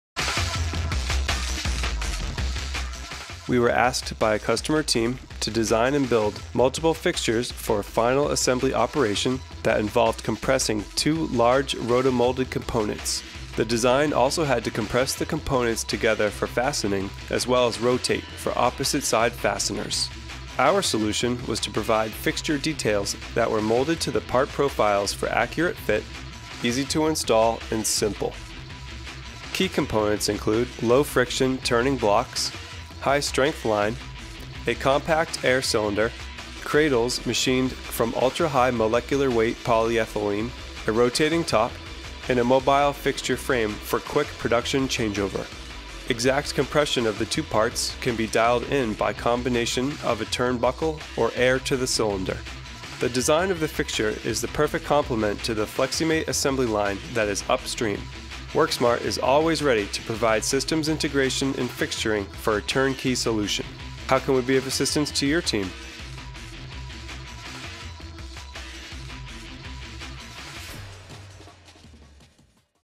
Lean Assembly Fixture
This custom application was designed for a final assembly operation that involved compressing two large rotomolded components. The design includes multiple fixtures that compress the components together for fastening as well as rotate for opposite side fasteners. The design of the fixture is the perfect complement to the Fleximate assembly line that is upstream.